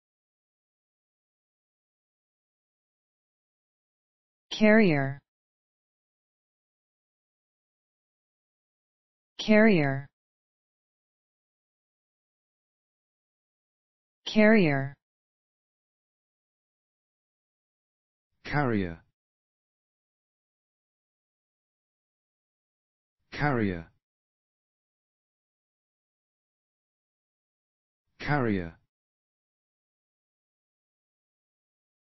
How to Pronounce carrier in American English and British English
Learn how to say carrier with HowToPronounce Free Pronunciation Tutorials.

This video shows you how to pronounce carrier correctly in American English and British English.